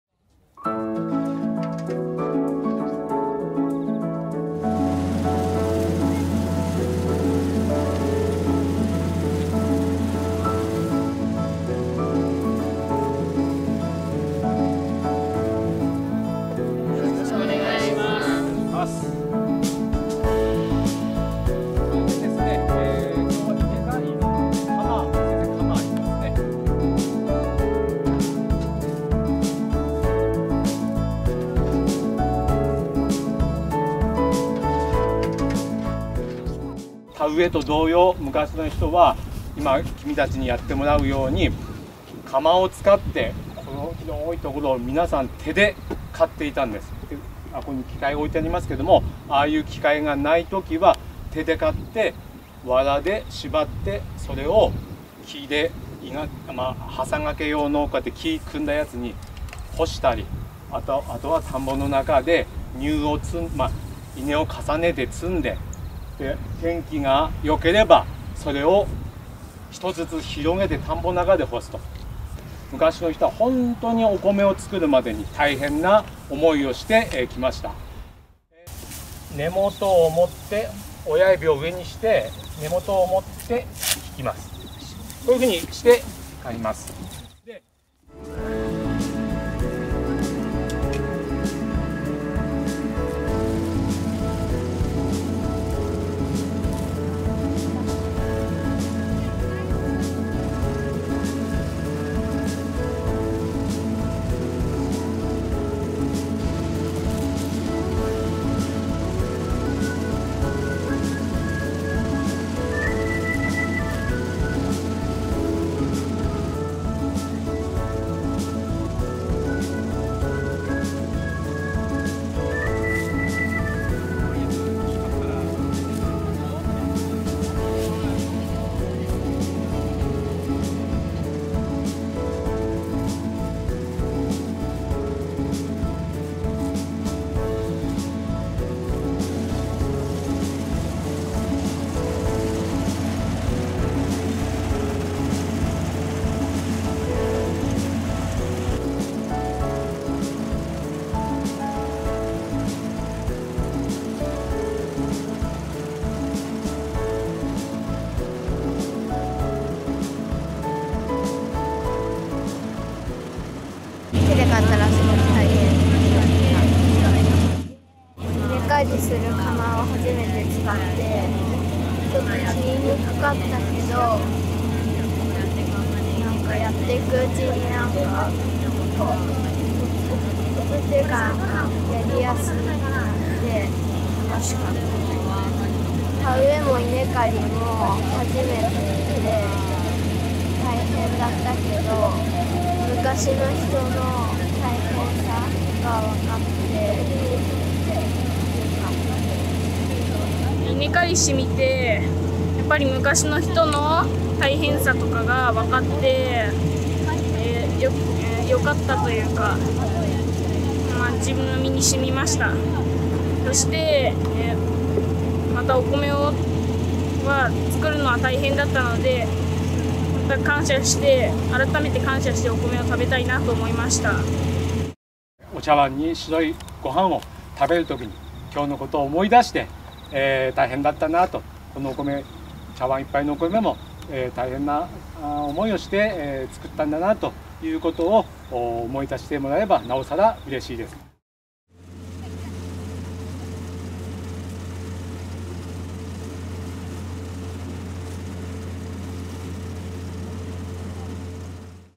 小学生が手刈りで稲刈り体験・立山小学校
立山小学校の５年生が、今年５月に田植えをしたコシヒカリを手刈りで稲刈りをする体験をしました。この体験は子供たちに農業を身近に感じて興味を持ってもらおうと行われました。児童たちは、鎌で稲を丁寧に刈り取っていきました。稲刈りを体験した児童は、「稲刈りは初めてだったけど、昔の人の大変さがわかってよかった。」と話していました。